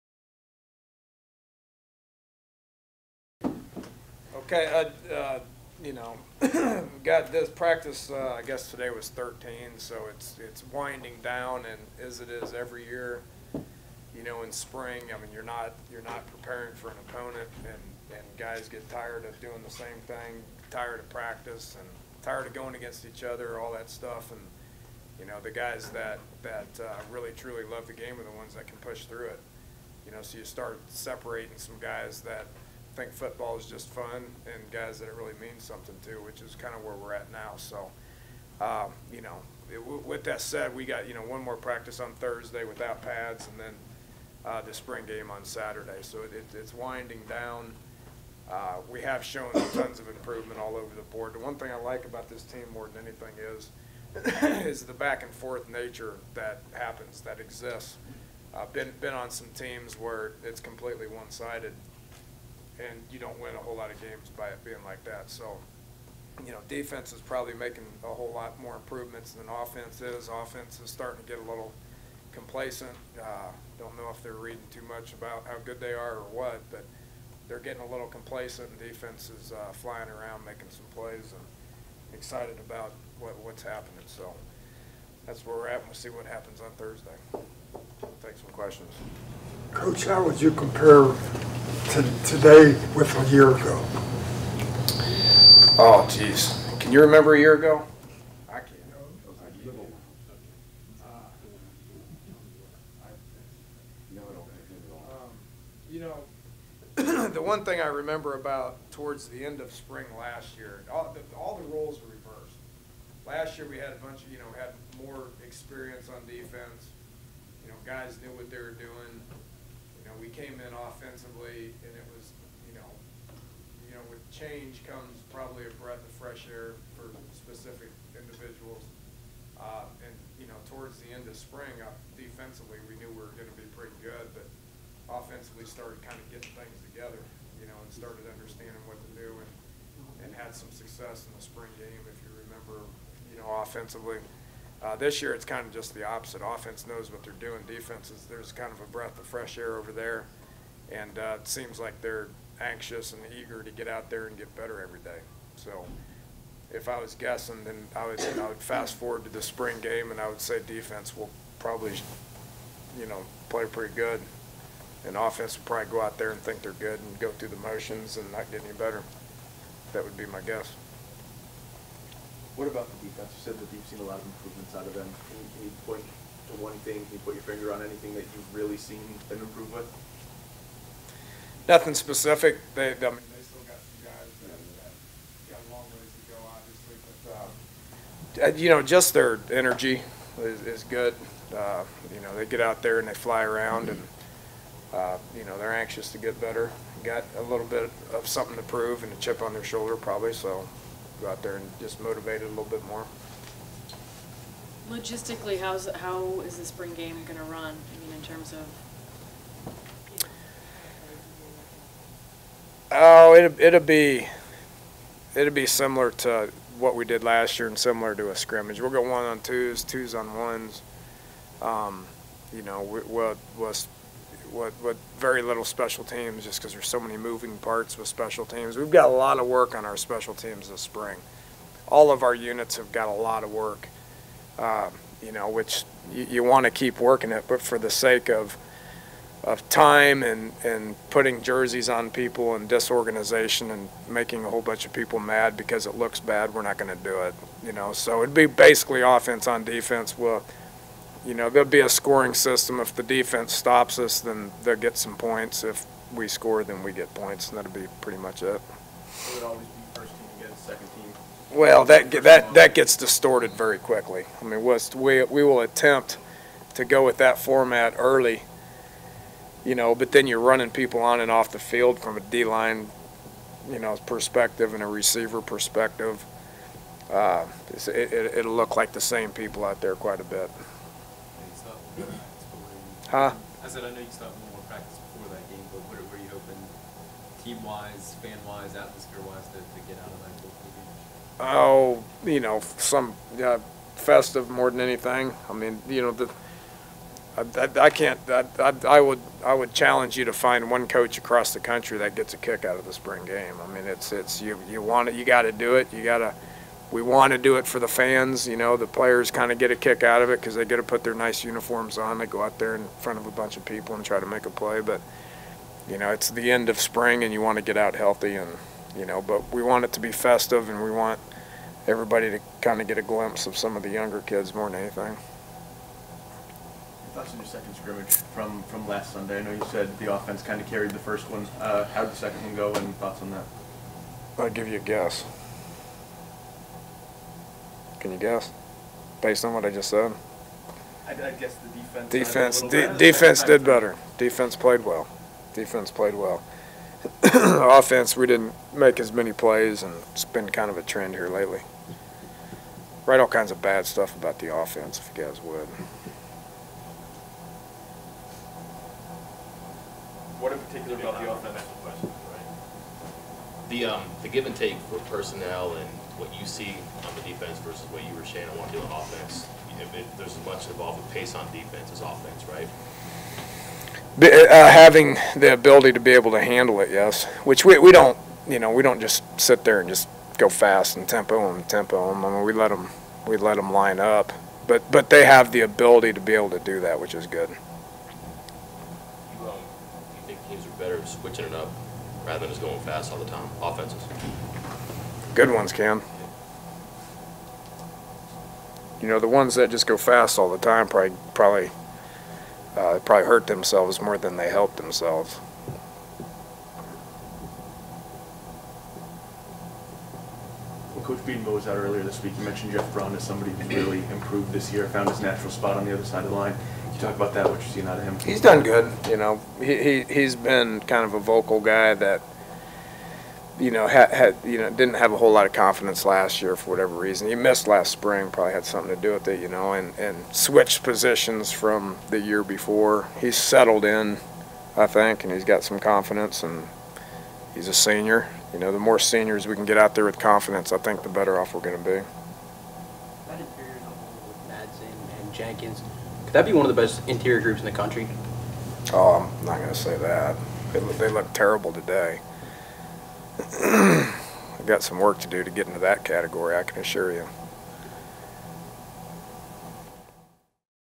Head Coach Dana Holgorsen
West Virginia coach Dana Holgorsen's Tuesday evening news conference following practice. April 17, 2012.